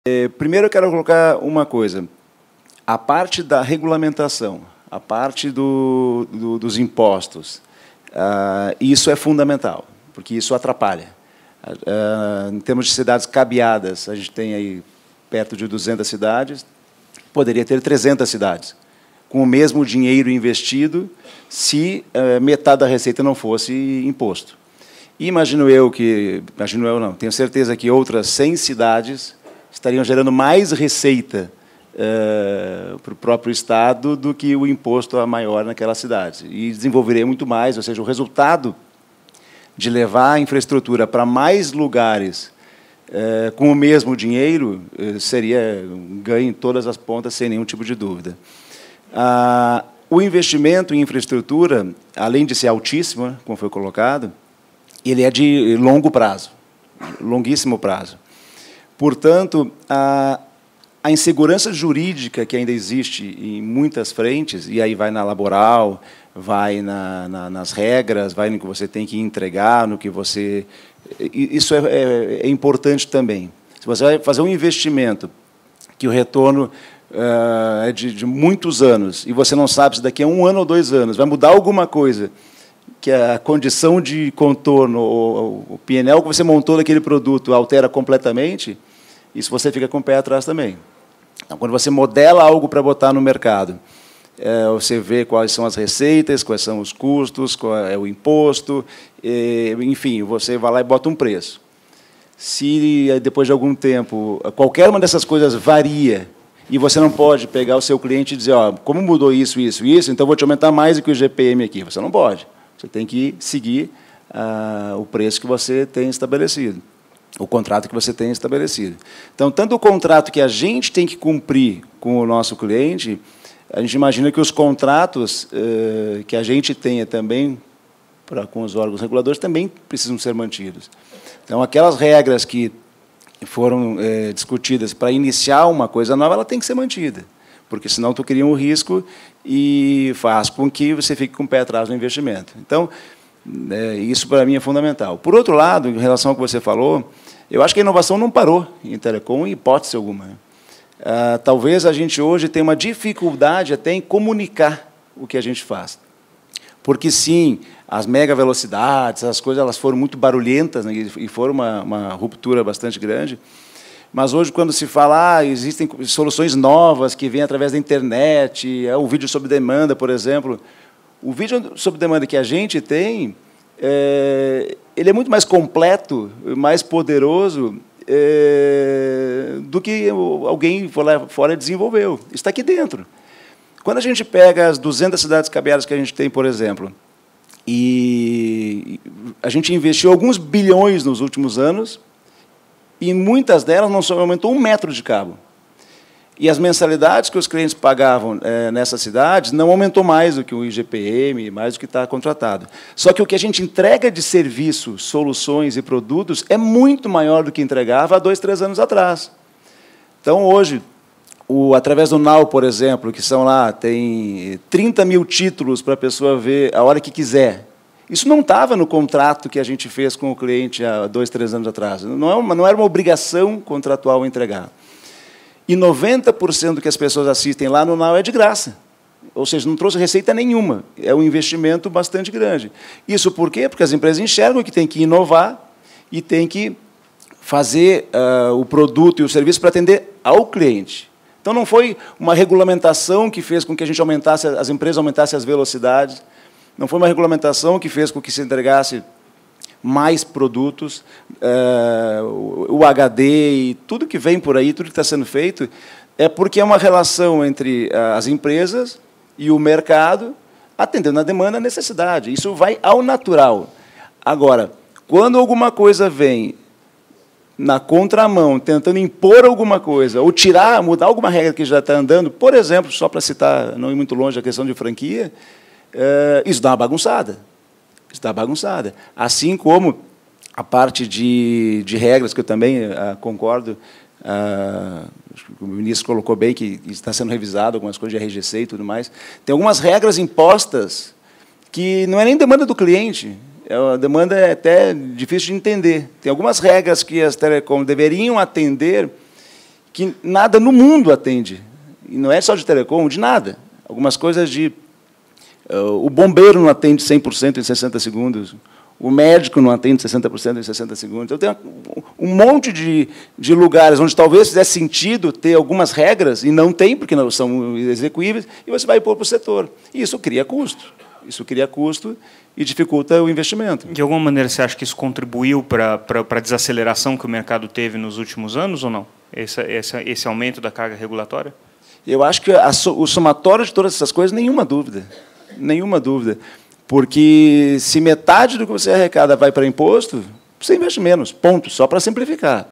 Investimentos em redes caem no Brasil com impostos altos
Surpresas na regulação e a escalada tributária estão derrubando o setor de telecomunicações. Como destacou o presidente da Net, Daniel Barros, durante o Painel Telebrasil 2016, ao buscar maior arrecadação, na prática o Estado promove o contrário ao inibir a ampliação da base.

“A questão tributária é fundamental, porque atrapalha. Temos perto de 200 cidades cabeadas, mas poderiam ser 300 com o mesmo dinheiro investido se metade da receita não fosse imposto. E essas 100 cidades estariam gerando mais receita que os impostos para o Estado. Levar infra para mais lugares com o mesmo dinheiro seria ganho em todas as pontas”, afirmou Daniel Barros.

Como insistiu, os investimentos em infraestrutura, além de elevados são de longo prazo. “Portanto a insegurança jurídica que ainda existe em muitas frentes – laboral, nas regras – é importante também. Um investimento com retorno em muitos anos sem saber se em um ano ou dois mudam as condições completamente, deixa de pé atrás. Assim como os contratos que temos com os clientes, a gente imagina que os contratos que a gente tem com os órgãos reguladores também precisam ser mantidas, senão cria um risco e faz com que fique com pé atrás no investimento.”

“Quando vem algo na contramão, como a questão da franquia, dá uma bagunçada. Se metade do que arrecada é imposto, investe-se menos. As incertezas regulatórias, novidades contra o que está contratado com o cliente, como a franquia, também. Mais de 90% dos clientes jamais tocaram no pico da franquia, então o mercado vai se ajustando. Agora, se diz que a partir de hoje não tem, ou vai entregar um serviço ruim ou torna o negócio inviável.” Assistam.